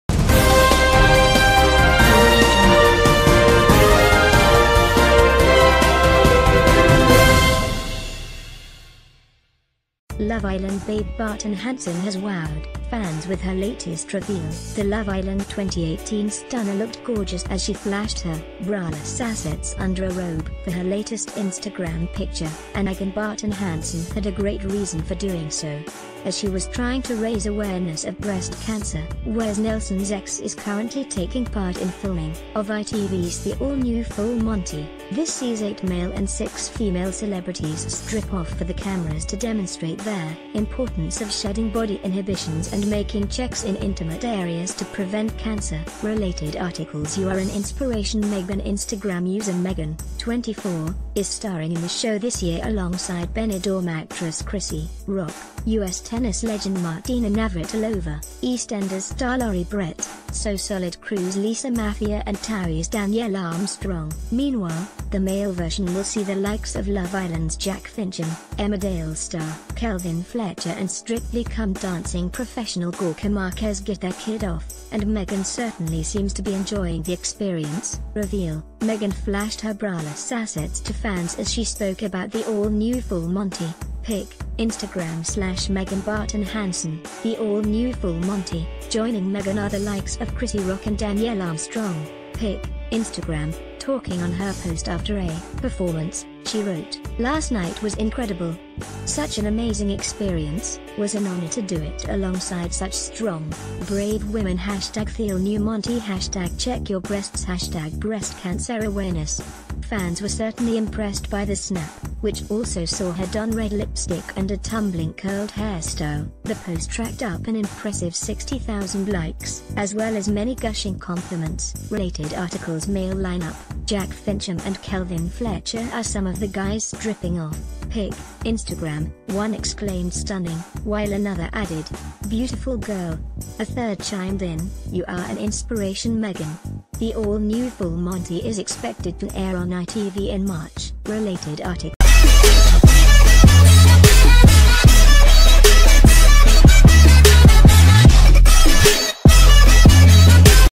Megan Barton Hanson flashes braless assets for the BEST reason
► LOVE Island babe Barton Hanson has wowed fans with her latest reveal. The Love Island 2018 stunner l...